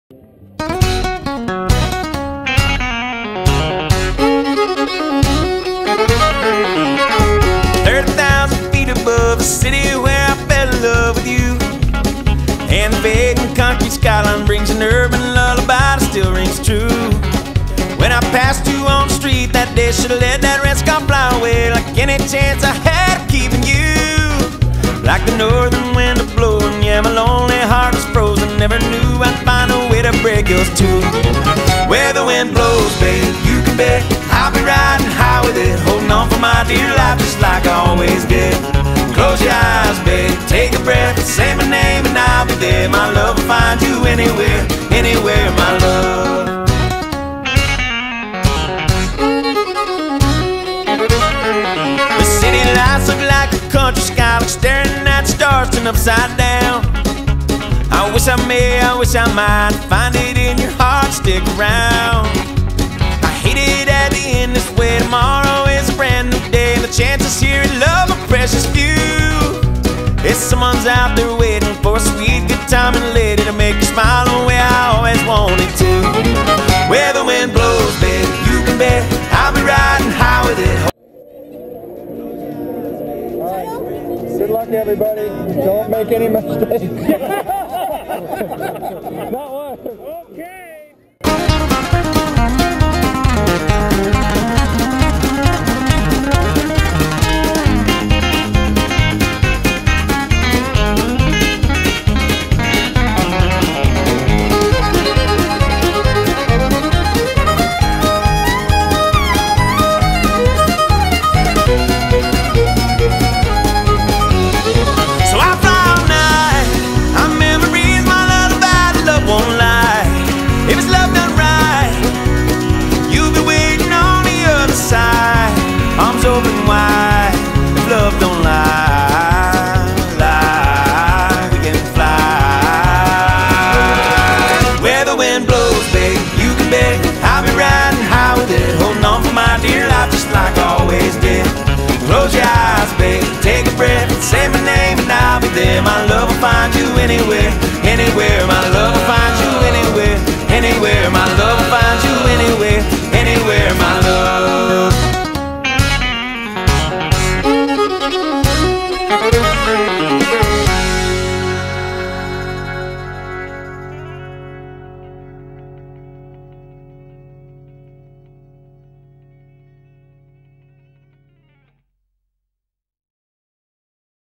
Factory Tuned Band @ Festival of Lights 2013
Behind the scenes sound check & first song...
Festival of Lights Nov 29th 2013 @ Main Stage Orange & Mission Inn